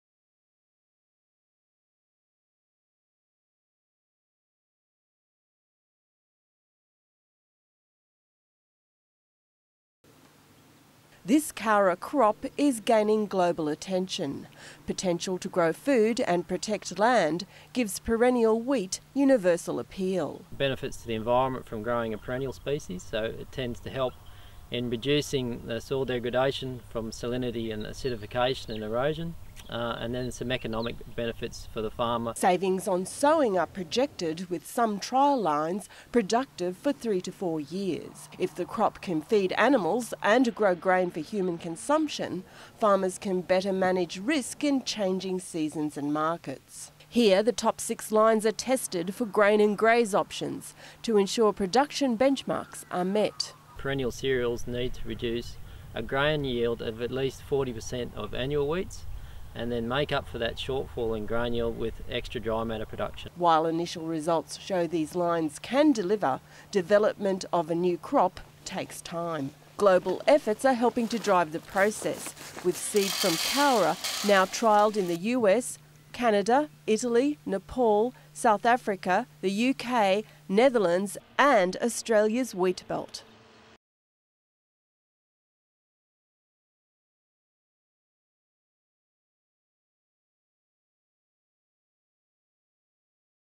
Perennial wheat stars on world stage
Global food security is inspiring interest in local perennial wheat research with the latest results from the Department of Primary Industries on show in Sydney this month at the 2013 International Grassland Congress.

It's a long-term project but one that has the potential to deliver a new crop with benefits for local producers and world-wide food production.